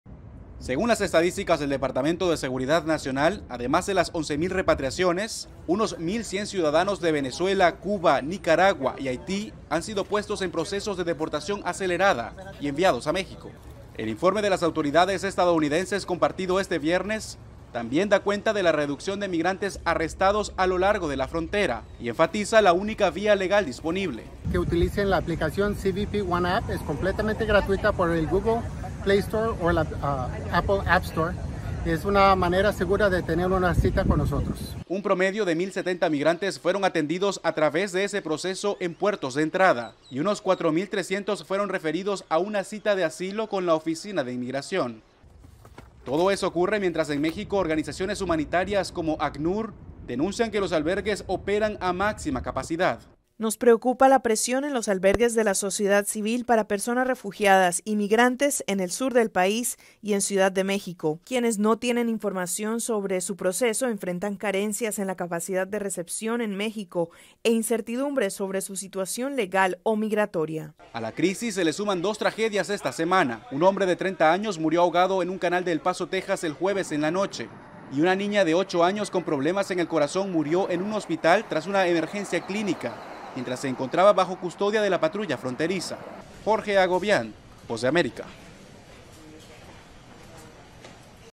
EUA efectuó 11.000 deportaciones en la primera semana del Título 8
Más de 11.000 migrantes han sido deportados a más de 30 países tras cruzar de manera irregular la frontera sur de Estados Unidos, a tan solo una semana de la entrada en vigencia del Título 8, según cifras oficiales.